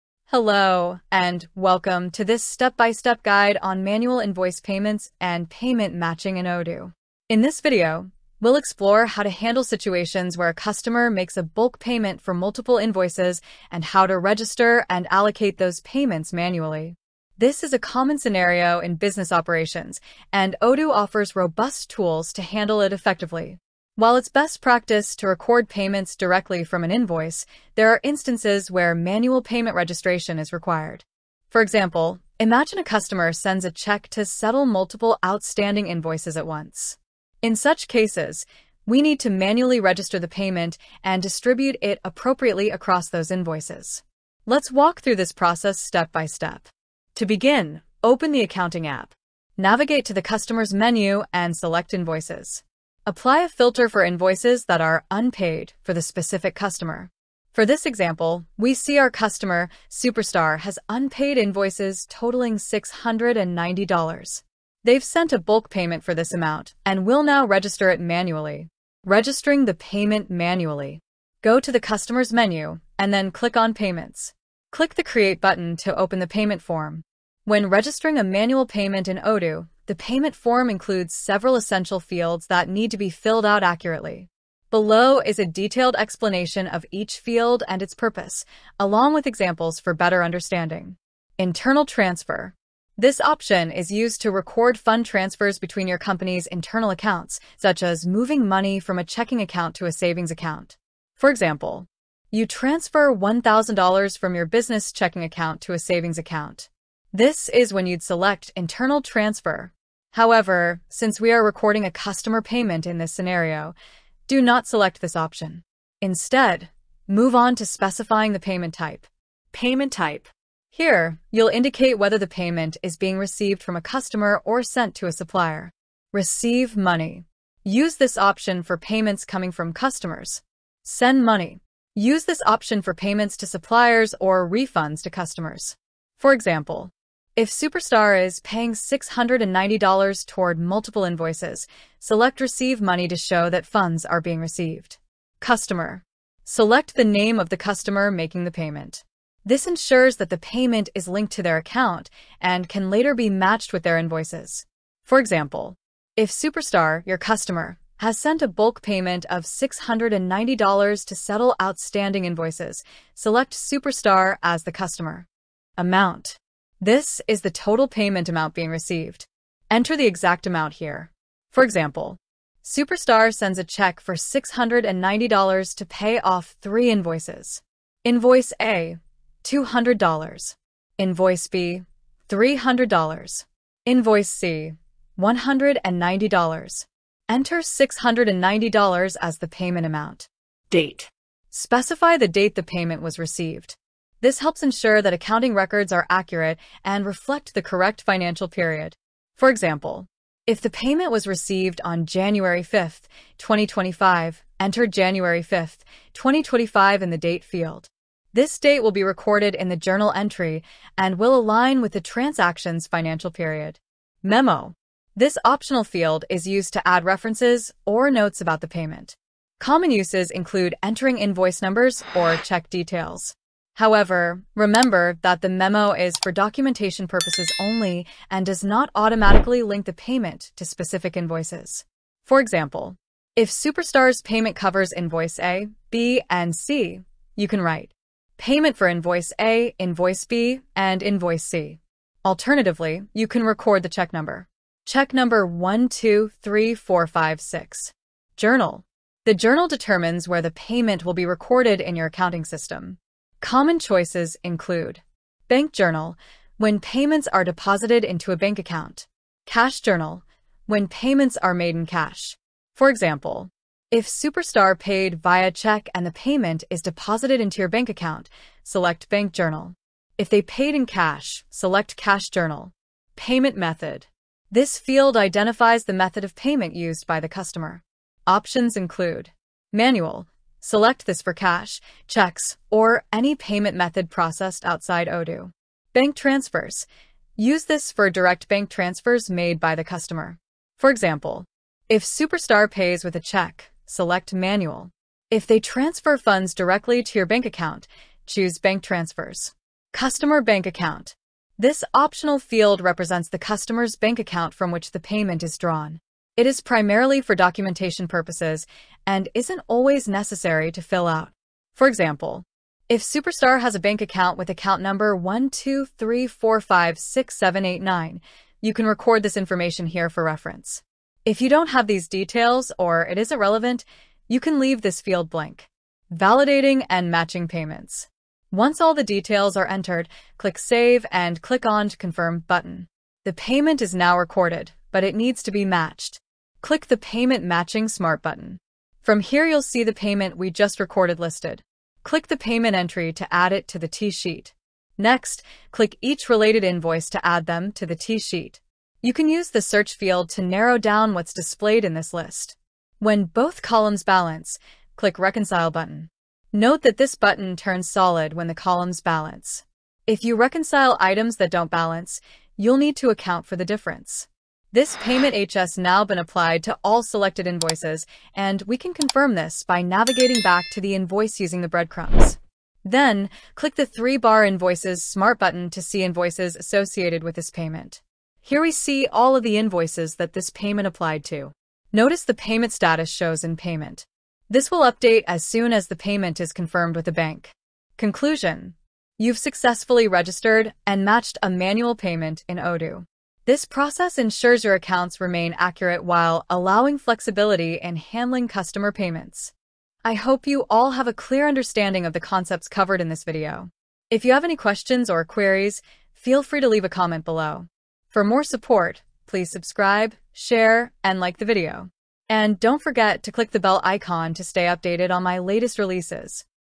Manual Invoice Payments and Payment Matching | Odoo Accounting
Learn how to manually record invoice payments and match payments to multiple invoices in Odoo. In this step-by-step tutorial, we walk you through handling bulk payments, filling out payment forms, and distributing payments across invoices. Perfect for businesses managing customer payments efficiently in Odoo.

🔗 Topics Covered:

    Recording manual payments in Odoo
    Matching payments to multiple invoices
    Explaining payment fields with real-life examples